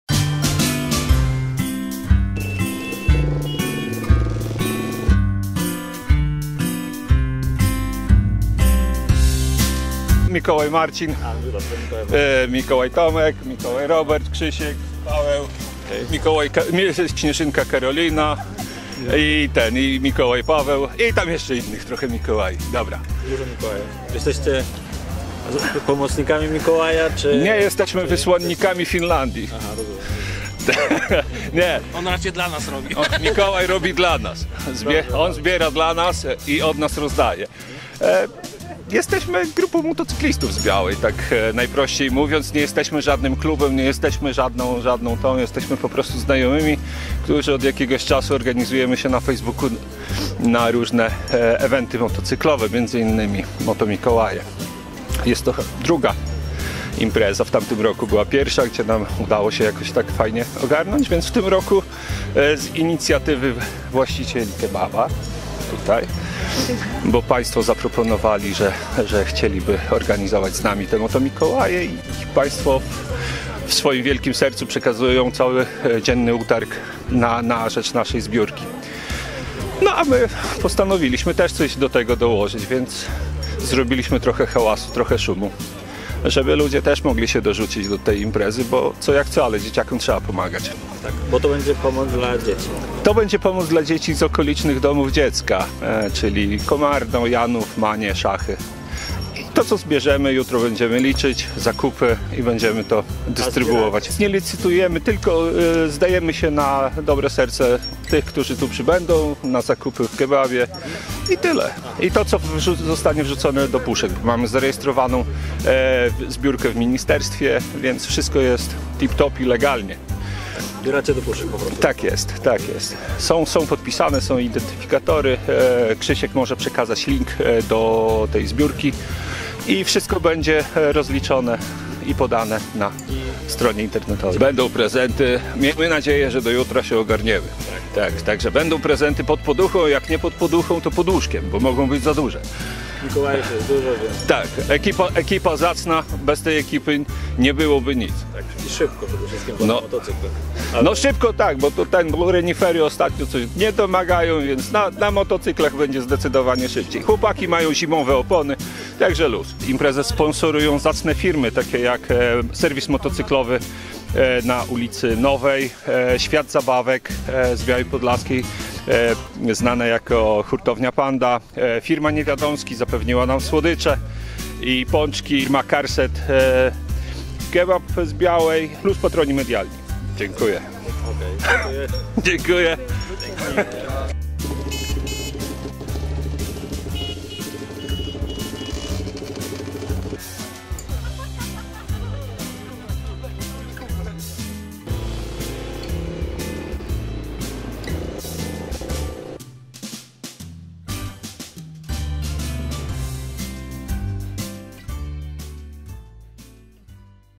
Biała Podlaska: Motomikołaje prowadzili zbiórkę na rzecz dzieci z domów dziecka
W niedzielę, 05 grudnia 2021 motocykliści z Białej Podlaskiej zorganizowali zbiórkę rzeczy dla podopiecznych okolicznych  domów dziecka. Akcja odbywała się w godz. 12:00-15:00 na placu przy ul.  Jana III Sobieskiego w Białej Podlaskiej - naprzeciwko stacji paliw Shell.